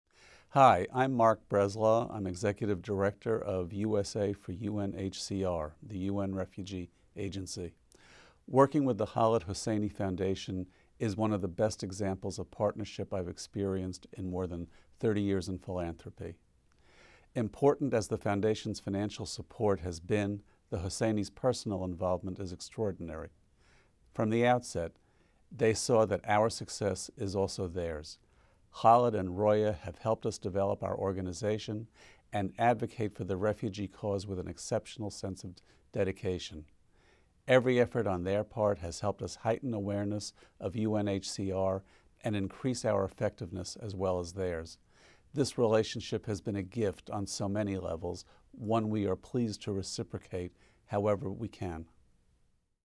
TKHF_Marc Breslaw.mov
Mark Breslaw is the Executive Director of USA for UNHCR.  In this video, he highlights the good work being done by The Khaled Hosseini Foundation.